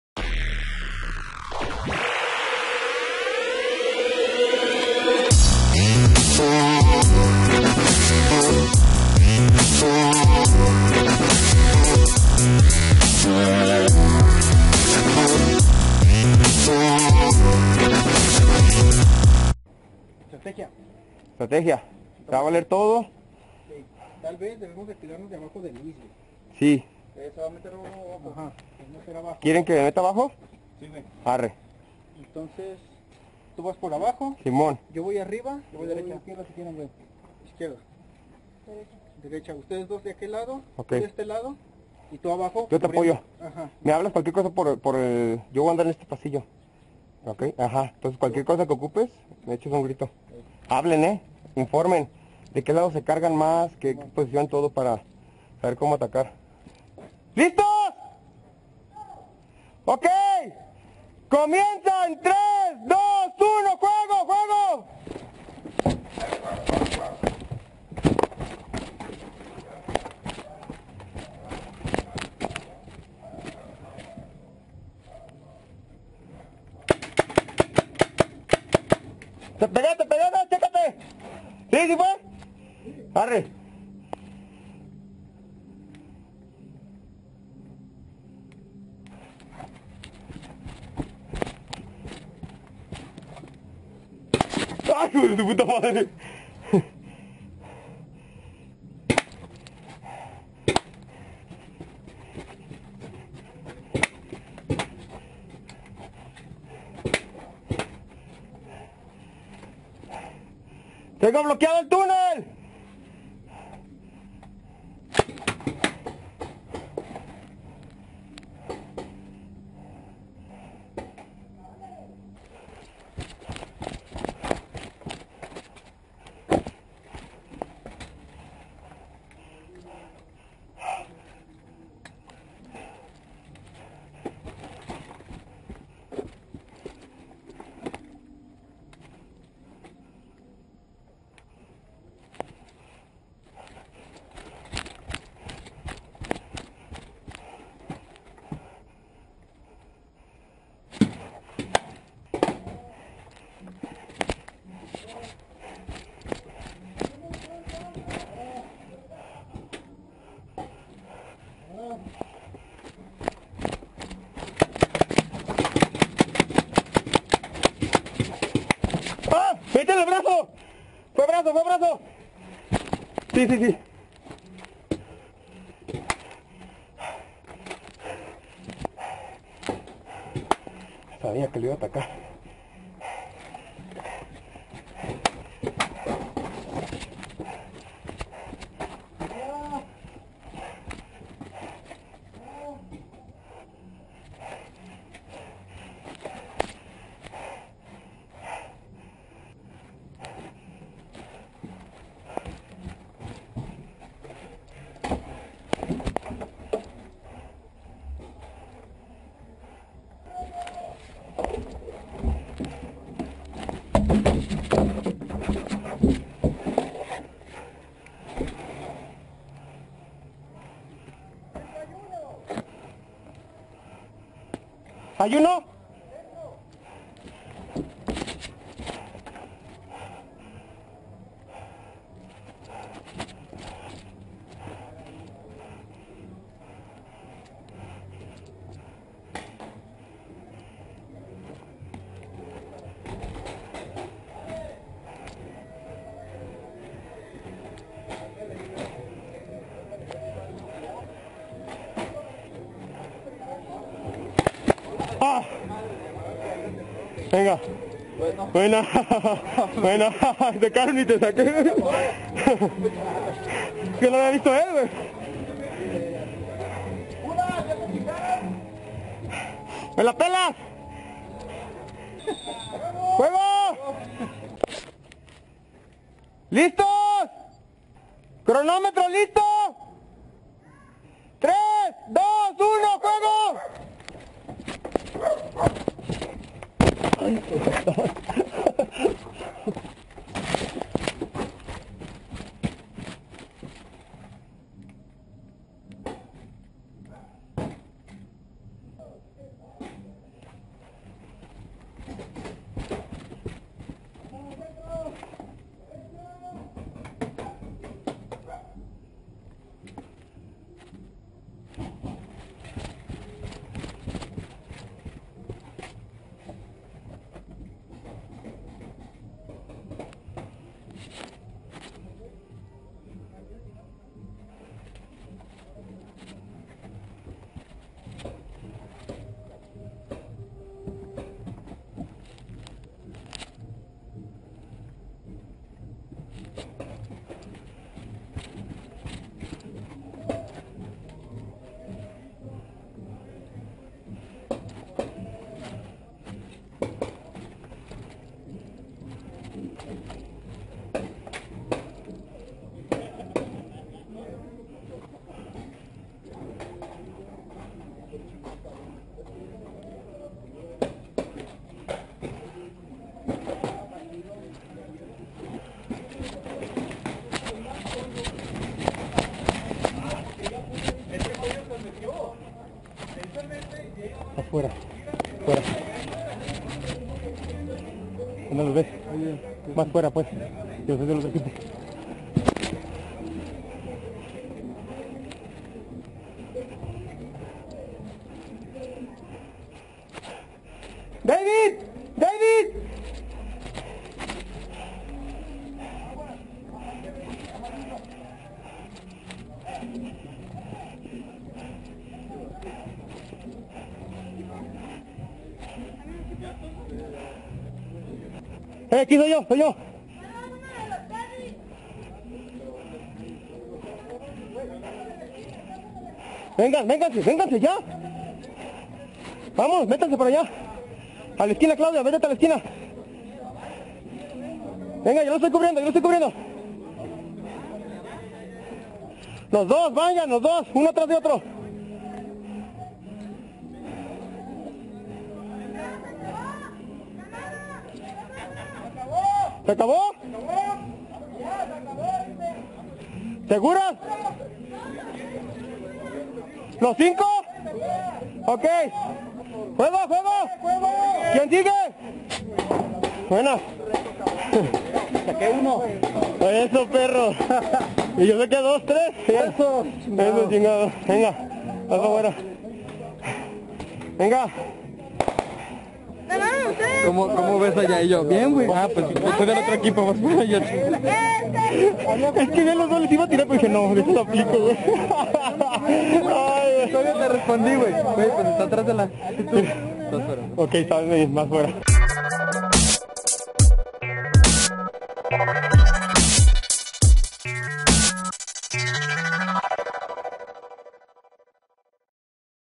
BATTLEFIELD PAINTBALL | POSADA G.F.E.S. | MAGFED PAINTBALL
Celebrando la posada del equipo en compañia de mas amigos para tirar pintura, excelente fin de semana sin duda.